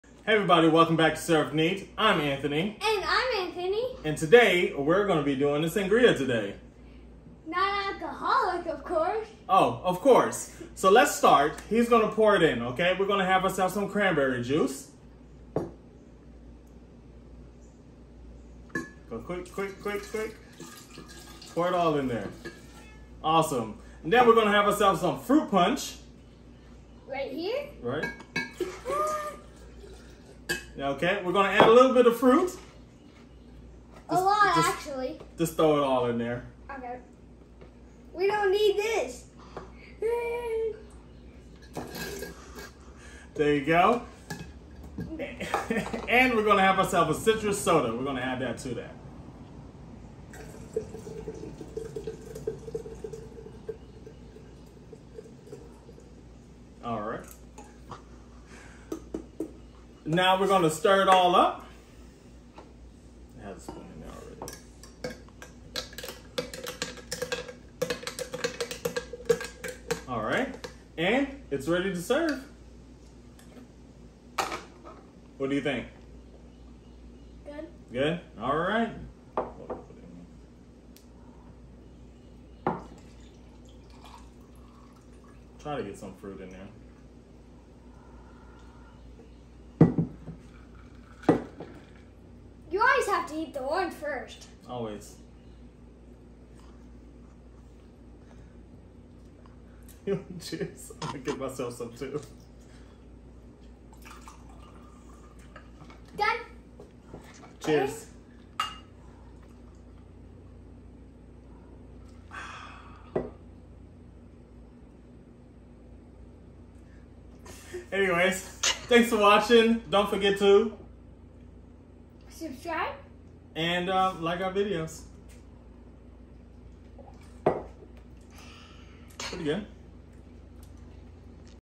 Take Your Kids to Work Day
Join Lil A and Big A as they make a kid-friendly fruit punch with a twist.  All you need is juice, fruit, and a lemon-lime soda!  Don't worry if it gets a little messy!